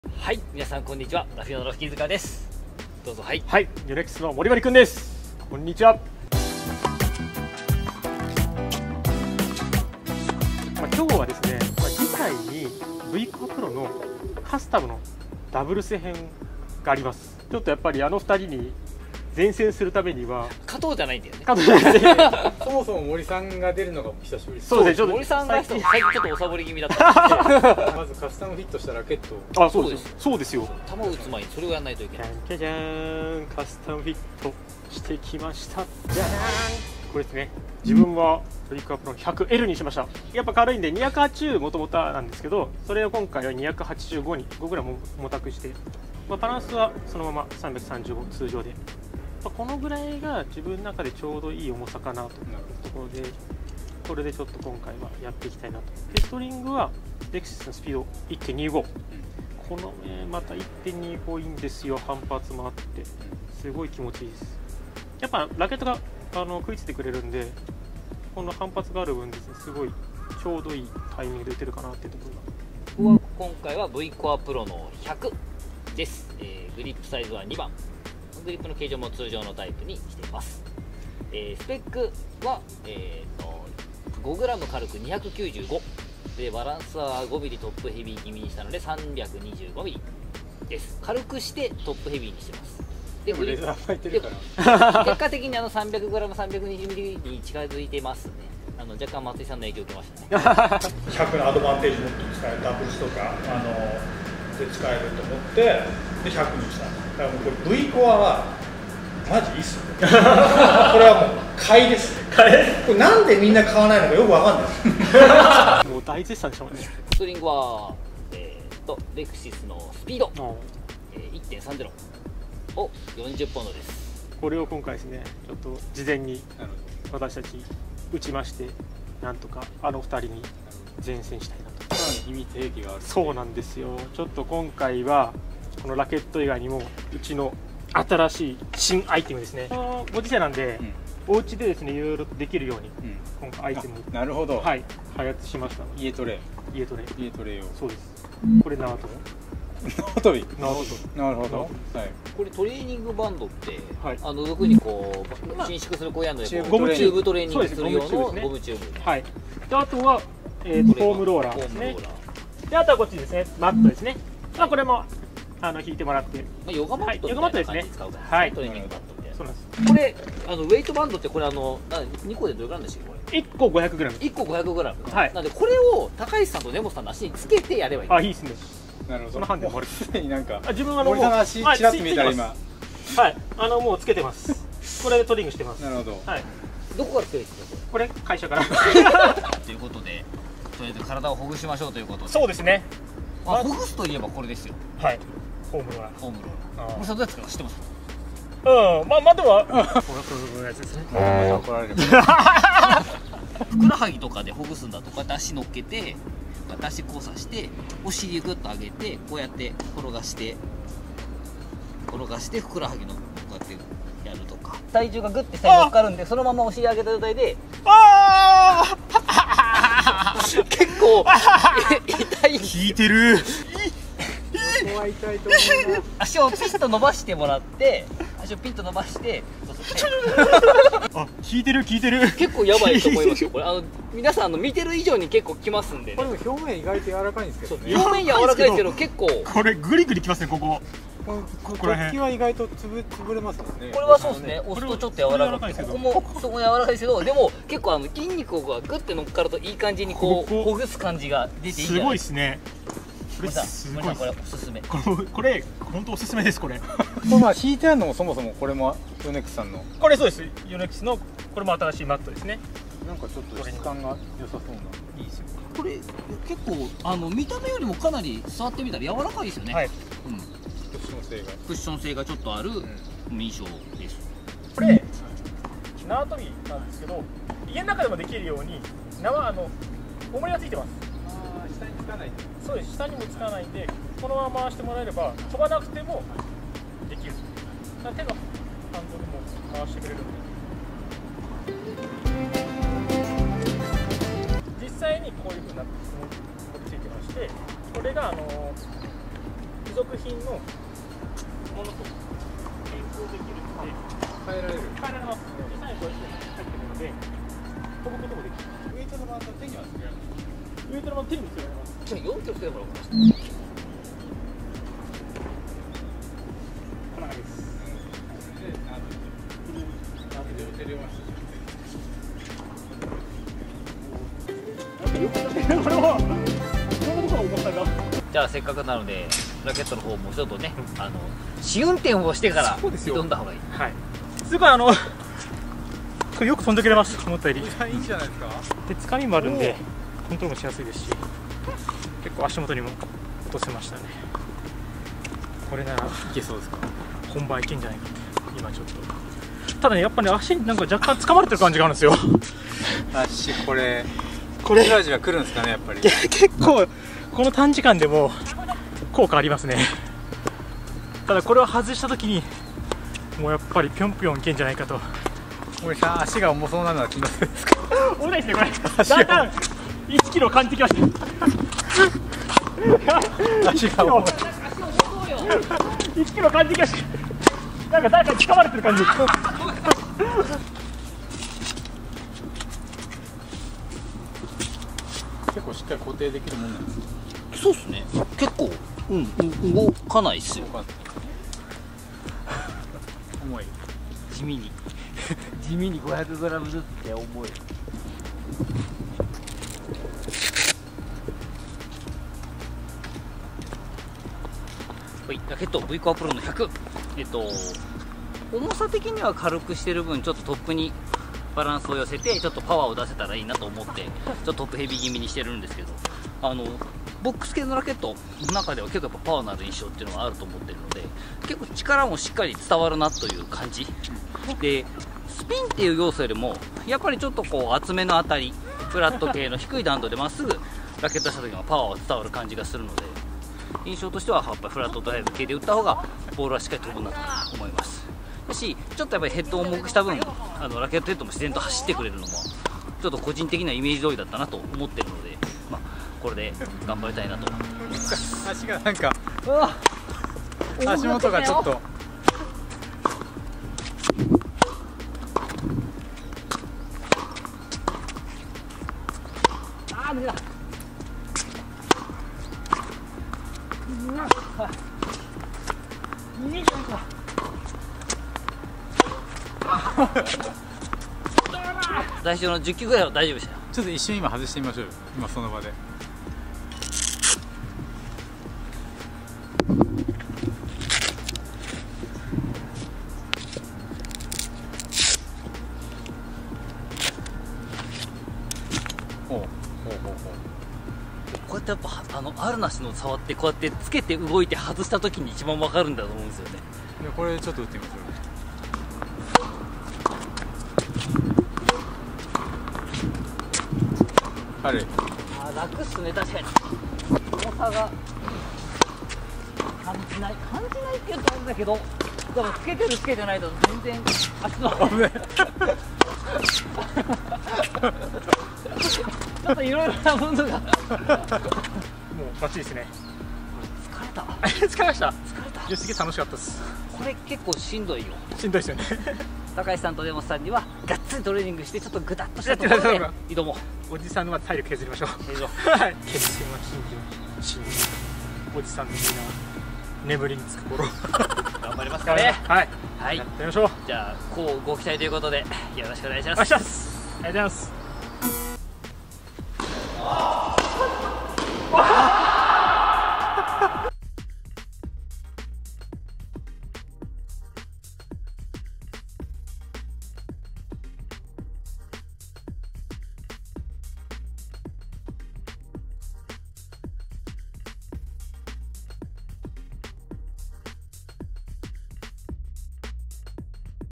【Fukky'sインプレ】ヨネックス『深夜のダブルス対決』にモリモリくんが復帰！！
ヨネックス恒例の深夜ダブルス対決にモリモリくんが電撃復帰！！VCORE PRO最新モデルをカスタムフィット、さらに新トレーニングアイテムで決戦へ向けて準備中です！！
【YONEX】
・VCORE PRO
・ストレッチカバー AC544 (テニス/ソフトテニス用)